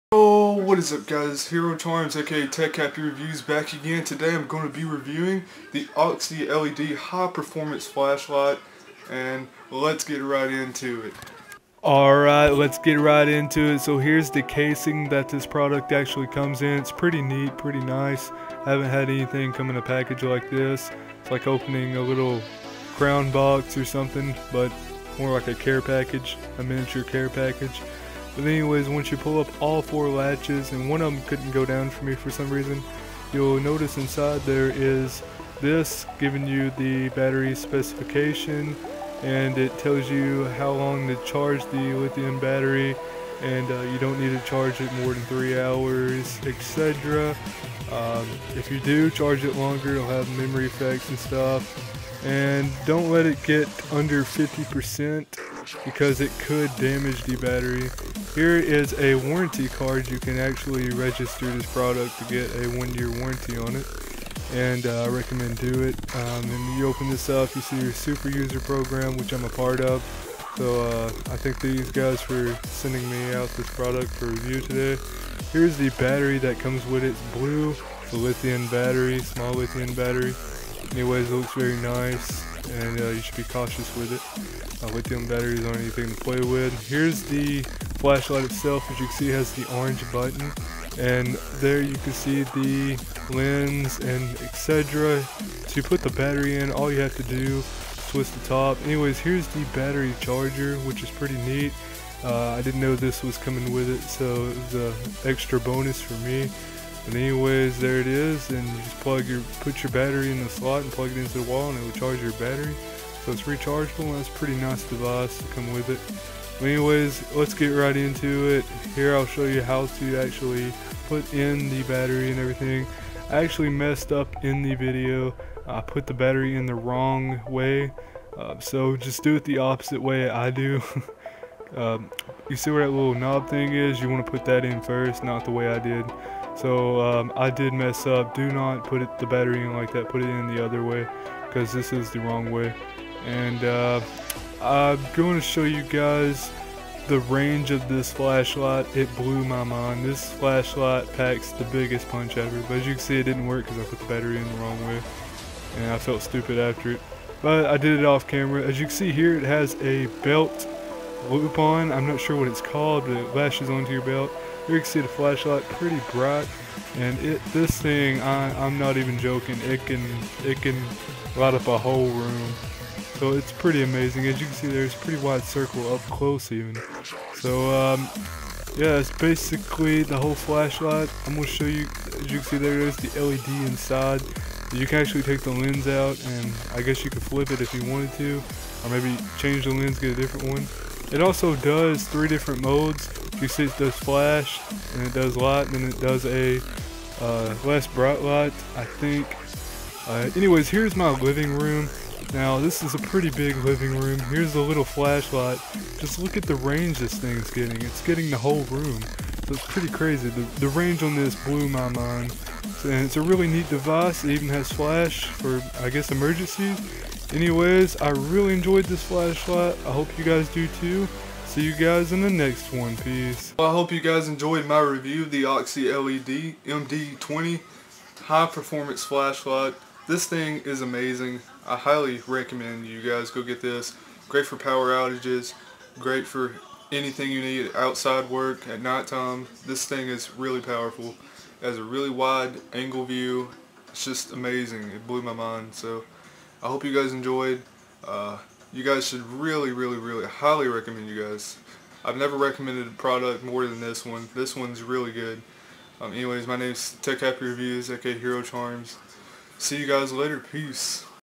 OXYLED High Performance Rechargeable MD20 Flashlight Unboxing/Review
OXYLED High Performance Rechargeable MD20 Flashlight Unboxing/Review Most amazing flashlight ive ever seen! i was amazed by how much power this bad boy packed! it was so crazy and for 16$ its worth it! i highly reccomend you guys go pick it up heres the link to purchase it :)